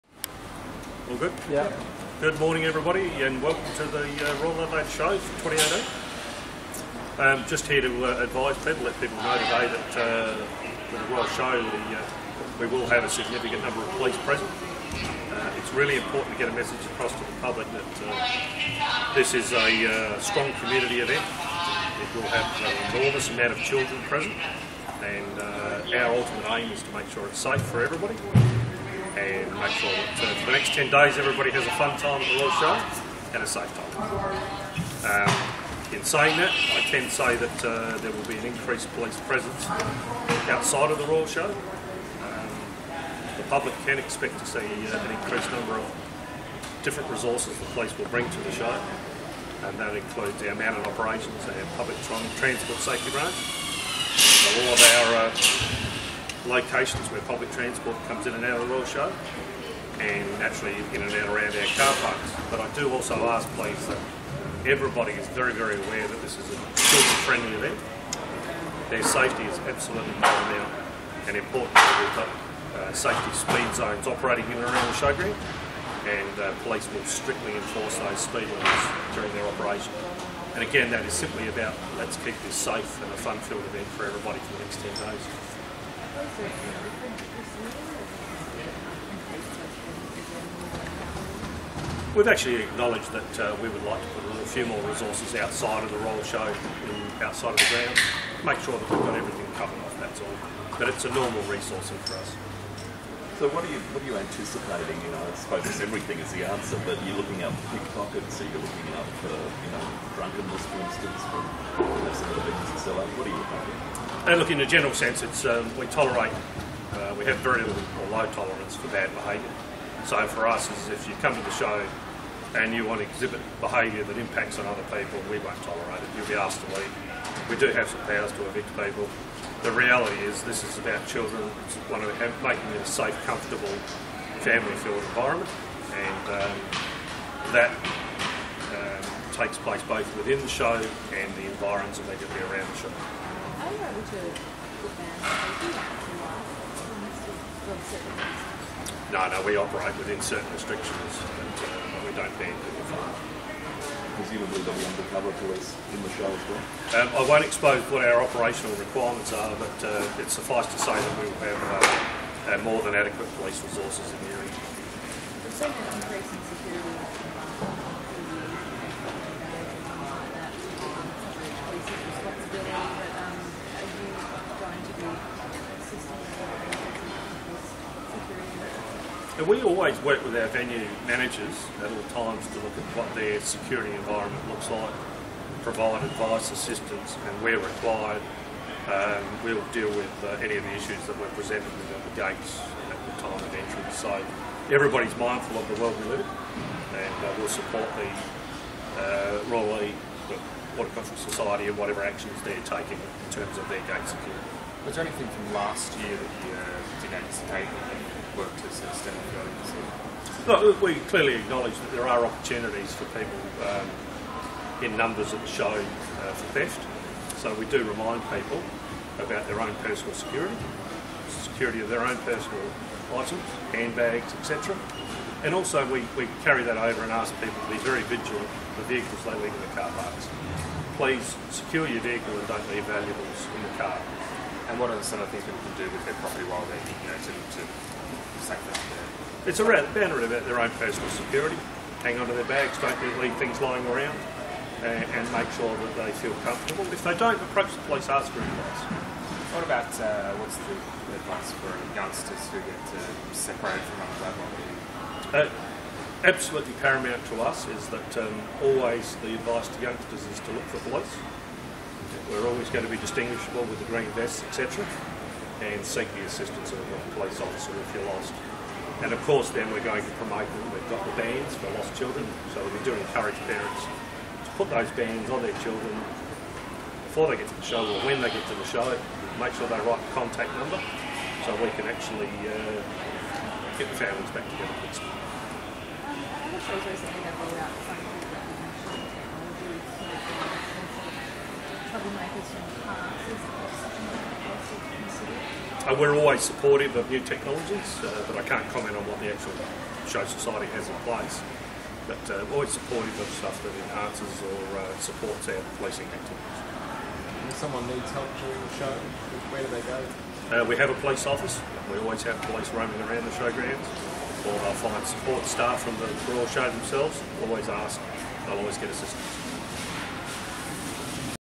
Chief Inspector Matt Nairn speaking at the launch of Operation Royal Show 2018
Attracting some 500,000 visitors across 10 fun-filled days, the Royal Adelaide Show is South Australia’s largest event - SA Police are committed to ensuring it’s also the safest, deploying a highly-visible and targeted presence both within and around the Showground.

Chief Inspector Matt Nairn, Police Commander for this year’s Royal Show, says SA Police are intent on safeguarding the family-friendly, inclusive atmosphere of the hotly-anticipated Show.

“The Show has always been a safe and welcoming environment for families. We have zero tolerance for aggressive or antisocial behaviour in this environment.

“Any person who is found to be causing a disruption or nuisance at the Show will be promptly evicted, potentially arrested and certainly prevented from re-attending.

“Don’t spoil the enjoyment of the Show for others - behave like there are small children watching.”

Police will enforce strict 25km/h zones around the Showground between 9am and 11pm. These zones will be clearly signed and apply daily on Greenhill Road, Goodwood Road and Leader Street.

Officers from SAPOL’s Public Transport Safety Branch will patrol buses and trains servicing the Showground, to help ensure that everyone gets to and from the event safely.

“We’ll be deploying officers outside of the Showground to help keep pedestrians safe, in addition to those members working out of our Show Police Station,” says Chief Inspector Nairn.

“Patrons can expect to see Mounted Operations and Dog Operations, in addition to uniformed and bicycle patrols in both the Showground and surrounding suburbs throughout the event.

Police are again urging parents to collect an ‘If I’m Lost’ wristband, designed to assist police in reuniting lost children and vulnerable persons with their caregivers. Parents are asked to clearly write their contact phone number, but not their children’s name on the wristband.

“Before departing, snap a quick picture of your children in what they’re wearing to the Show. This can greatly assist us in spotting lost children, and reuniting them with their caregivers”, says Chief Inspector Nairn.

Wristbands are available from all Show Kiosks, the BankSA Farmyard Nursery, Royal Show Office and Show Police Station, or can be obtained from any uniformed officers on duty at the Showground.

Lost children will be taken to the Show Police Station, located at the rear of the north-western grandstand of the Main Arena.

Parents with older children are encouraged to designate a meeting place before arriving at the Show, where families and groups can gather if they become separated from the group.

Royal Agricultural & Horticultural Society Chief Executive John Rothwell said the Royal Adelaide Show is a time where family and friends get together for a great day out.

“The Society has a strong commitment to safety in general and encourages all patrons to be aware and alert of their surrounds.”

With half-a-million people attending the Show annually and thousands of animals on-site at any one time show-goers do need to be mindful of what’s happening around them.”

There’s more than ever before from SA Police at this year’s Royal Adelaide Show:

Check out our brand-new ‘Beware What You Share’ virtual reality experience and ‘Road Safety Pledge’ photo booth at the SA Police display in the Jubilee Pavilion.

Visitors can have their photo taken on a SA Police BMW motorcycle for a gold coin donation.

Visitors can also learn more about Blue Light programs, Neighbourhood Watch and careers with SA Police.

The Band of the South Australia Police will be performing twice daily, between 11am and 11.45am (except Tuesday and Wednesday) and again between 5pm and 5.45pm at the Golden North Stage.

Additionally, the Rotary Club of Unley will be announcing the South Australia Police Officer of the Year on the Golden North Stage at 11.30am on Friday 7 September.